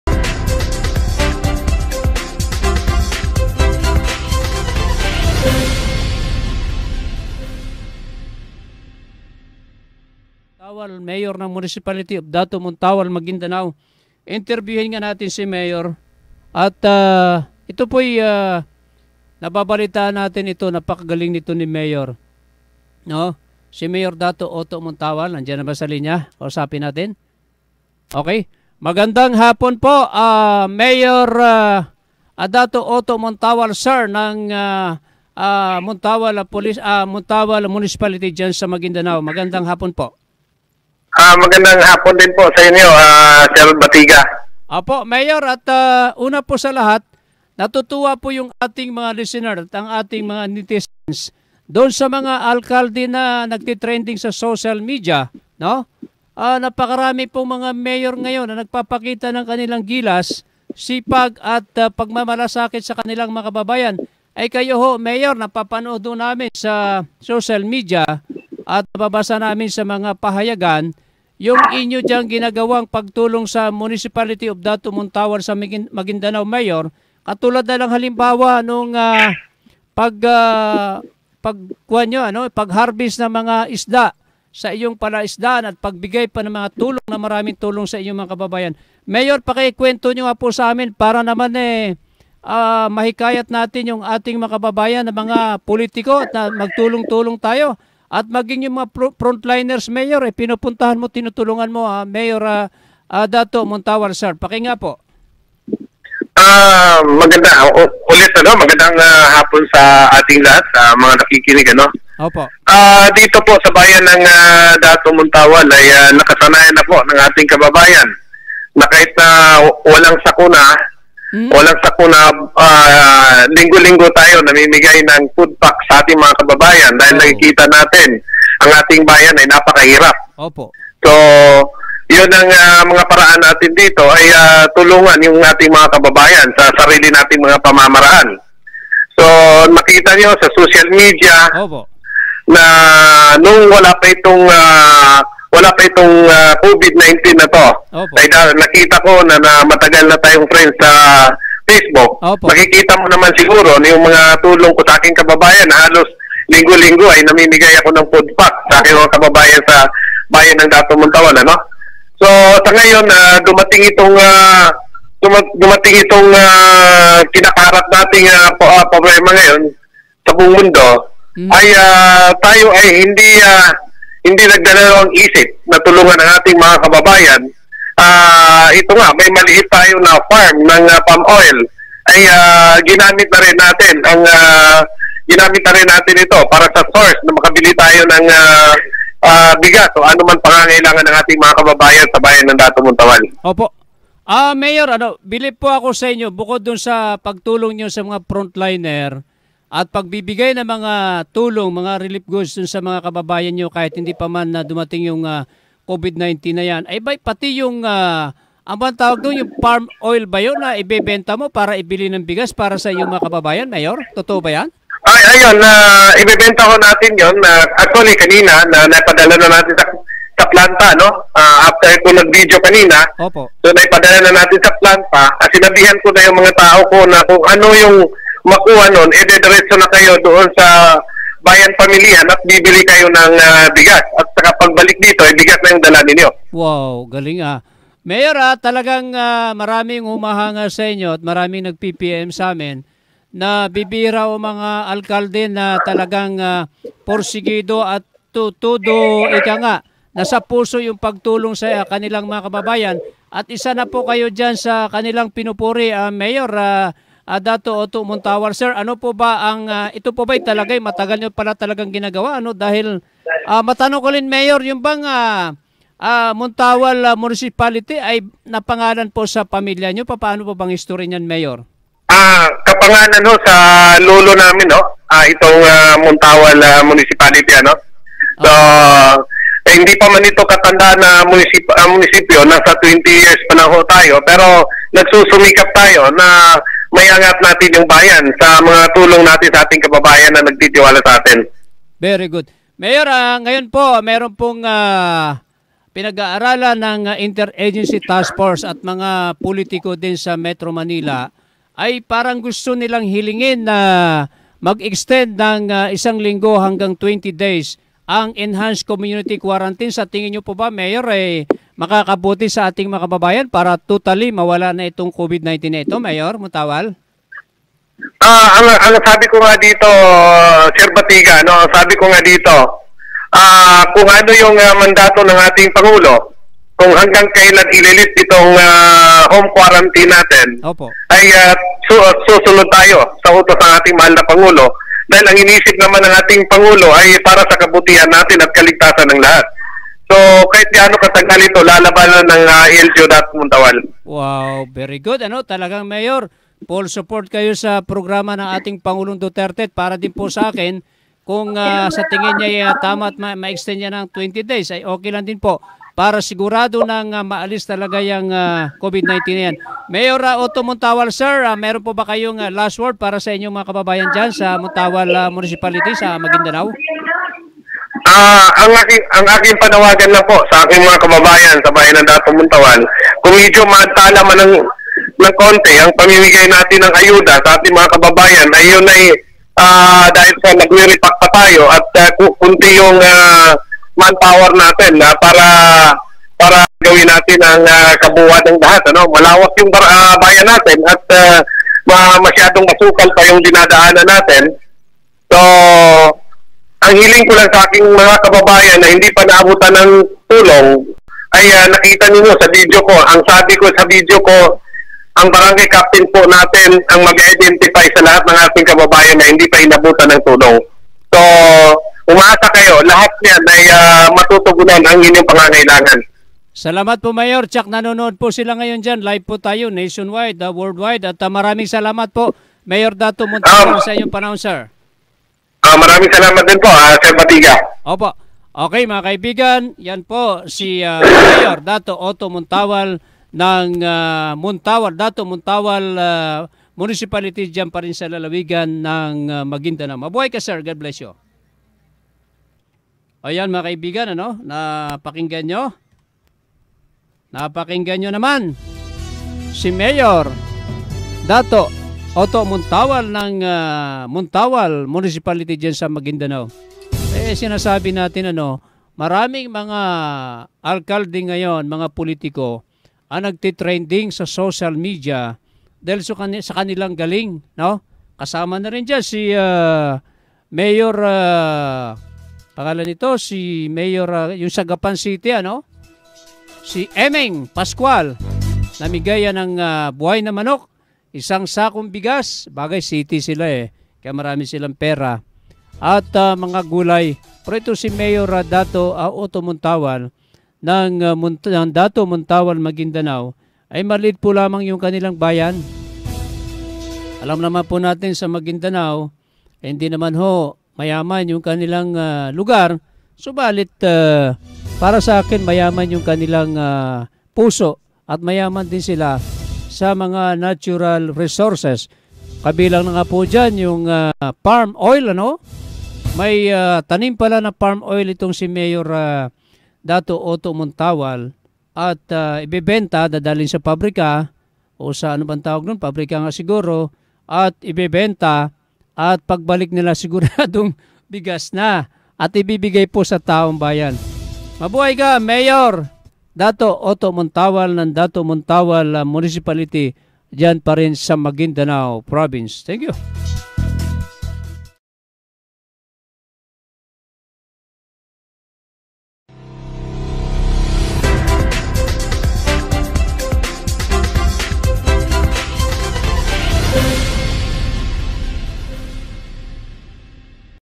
MAYOR DATU OHTO C. MONTAWAL NASA DUGO NA ANG PAGIGING MATULUNGIN SA KAPWA!
MAYOR  DATU MONTAWAL NASA DUGO NA ANG PAGIGING   MATULUNGIN SA  KAPWA|PAGBIBIGAY NG TULONG PATULOY SA BAYAN NG  DATU MONTAWAL (MAGUINDANAO PROVINCE)